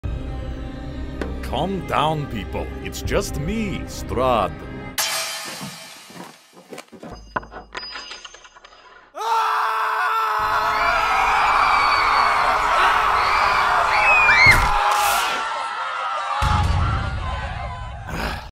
Strahd visits Vallaki
A clip from my upcoming series "I, Strahd". The series showcases the normal life everyone's favorite vampire lord. in this clip Strahd visits Vallaki and gets a warm welcome.